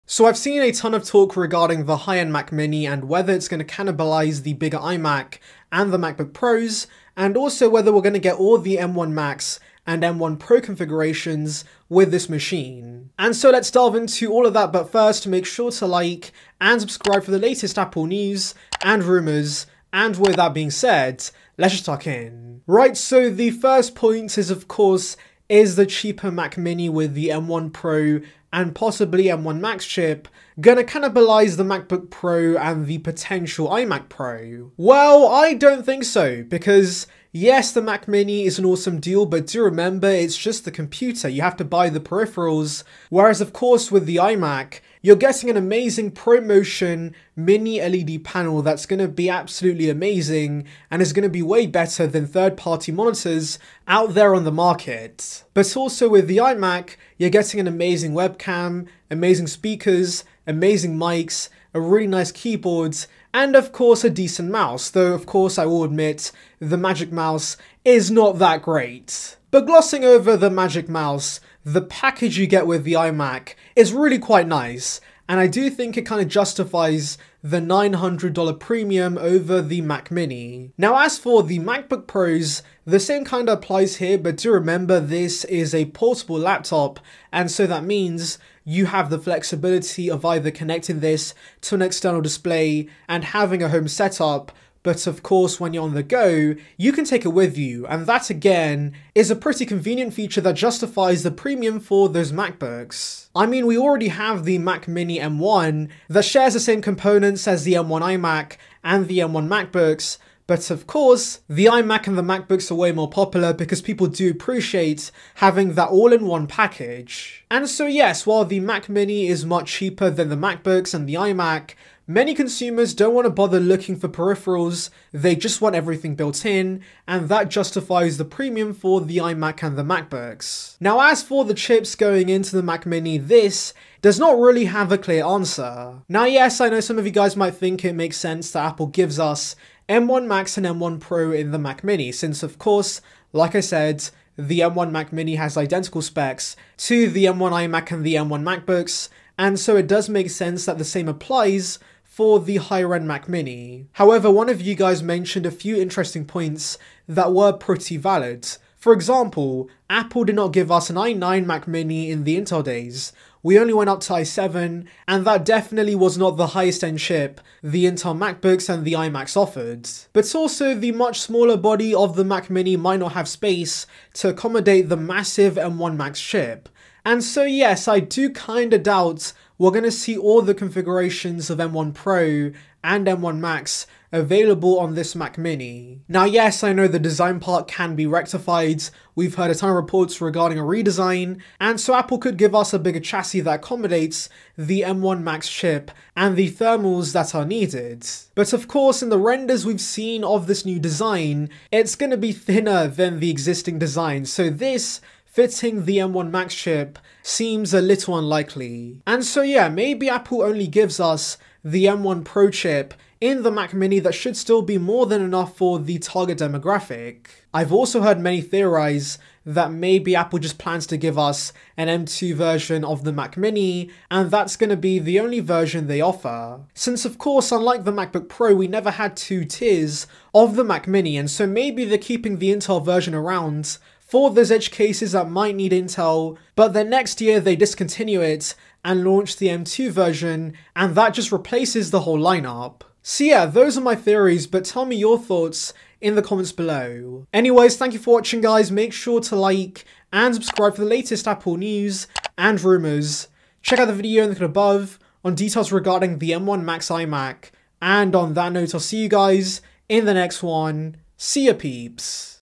M1 Pro & M1 Max Mac mini - NEW DETAILS!
Time-stamps:
Intro 00:00
Would The M1 Pro/Max Mac mini Cannibalise The M1 Pro/Max iMac and MacBook Pro? 00:22
Will All M1 Pro/Max Configurations Be Available To The Mac mini? 02:10
Could The 2022 Mac mini Be Only Available With M2? 03:28
Outro 04:02